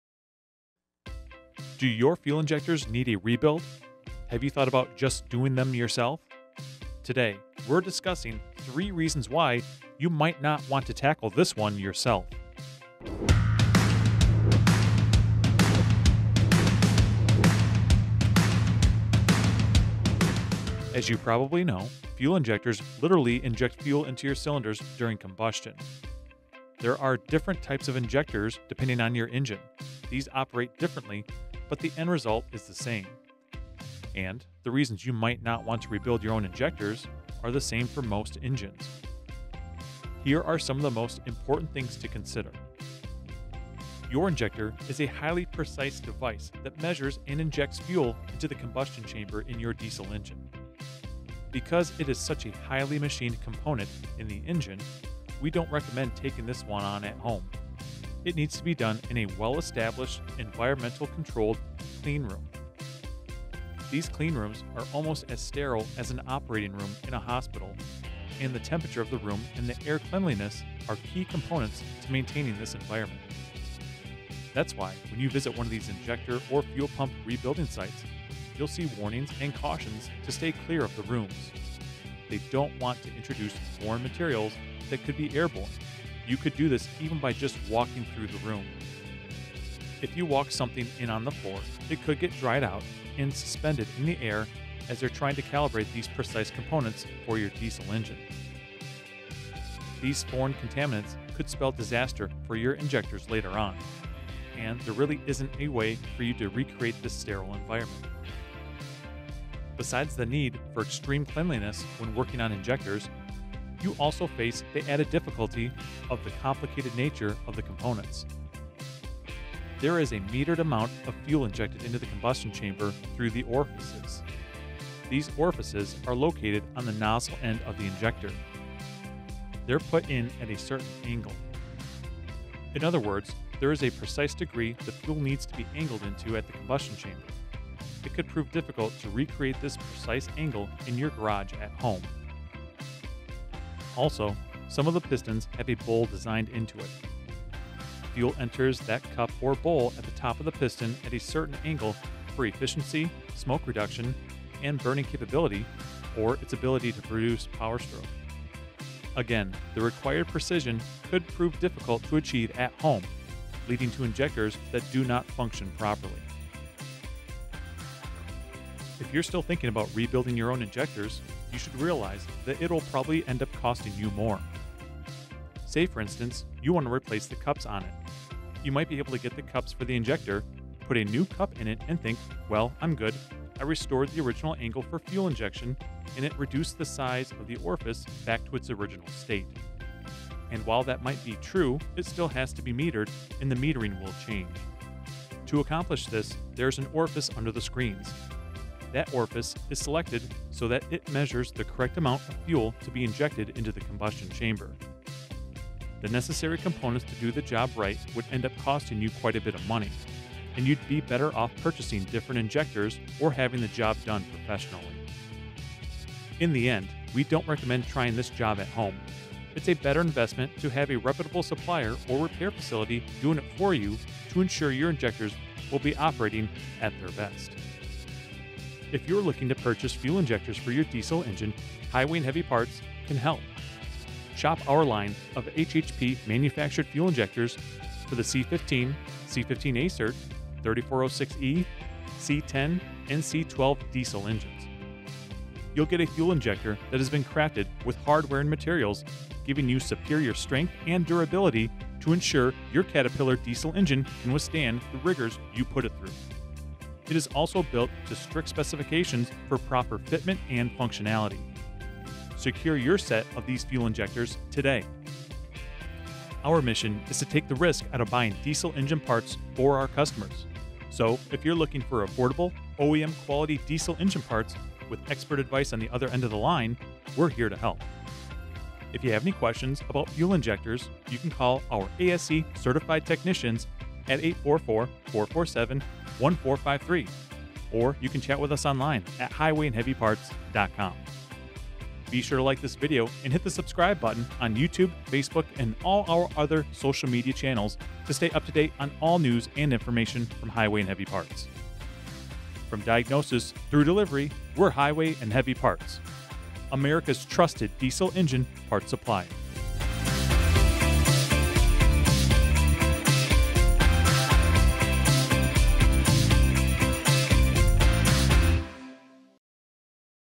Don't Rebuild Your Own Diesel Fuel Injectors!
Why You Shouldn’t Rebuild Your Own Diesel Fuel Injectors.

In today’s video, we’re discussing three reasons why you might not want to tackle rebuilding your fuel injectors yourself. Let’s get to it!

Step-by-Step Guide/Tutorial:
Precision Requirements: Fuel injectors are highly precise devices that need to be rebuilt in a clean, controlled environment, like an operating room, to avoid contamination.
Complex Components: The components of fuel injectors, including the metered fuel orifices, require precise angles and measurements that are hard to replicate at home.
Cost Considerations: Attempting to rebuild injectors at home can end up costing more than having them professionally rebuilt due to the need for specialized tools and parts.

What You Will Learn:
The importance of a clean environment for rebuilding injectors
The complexity of injector components
Cost implications of DIY injector rebuilds

Key Benefits:
Ensuring injectors function properly
Avoiding potential damage from improper rebuilds
Saving money and time by using professional services
Suggested Video Tags: Fuel injectors, injector rebuild, diesel engine maintenance, engine performance, DIY injectors, diesel parts, Highway and Heavy Parts